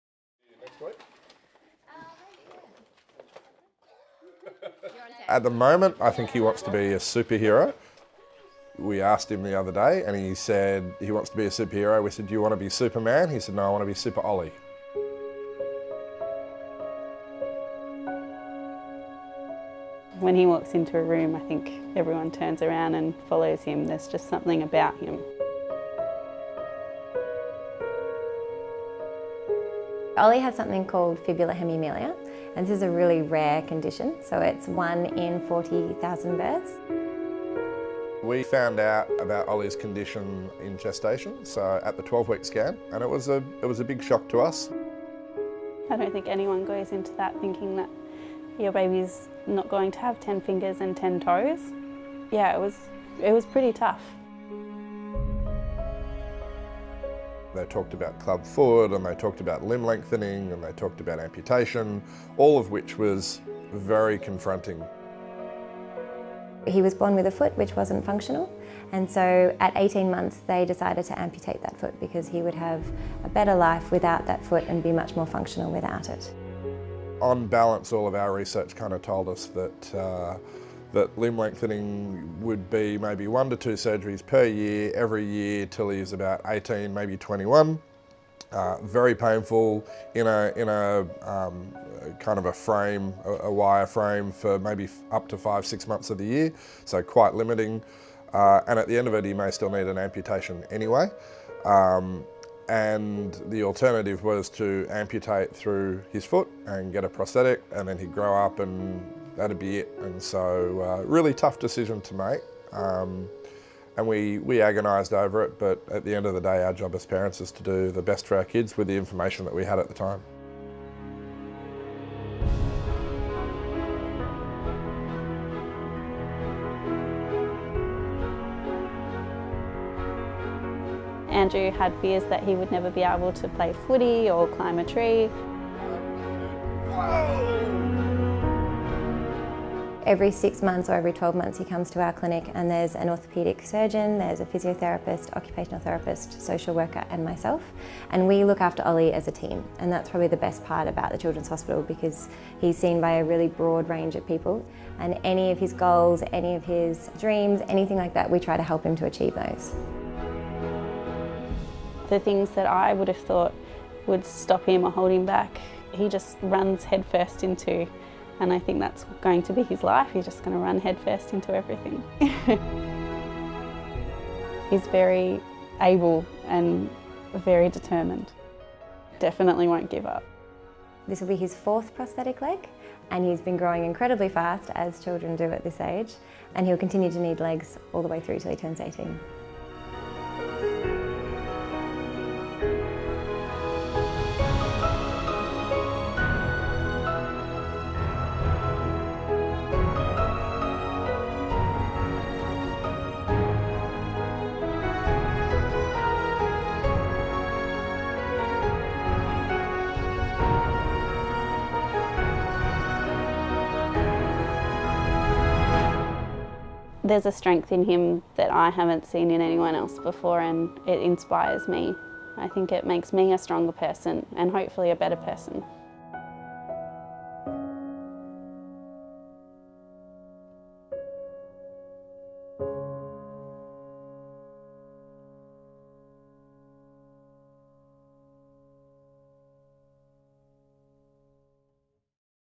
Meet Ollie, the face of the 2019 Good Friday Appeal
If you saw little Ollie running around, you would think he was like any other four year old. He’s charismatic, full of life and will go exploring at any opportunity he gets. But Ollie faces challenges every day after having the lower part of his leg amputated in 2016.

At only six weeks post-surgery at The Royal Children's Hospital, Ollie was fitted with his first prosthetic leg – and he hasn’t looked back since.

Thanks to your generous support, we can make a difference to children like Ollie and help The Royal Children’s Hospital continue to be a world leader in paediatric care.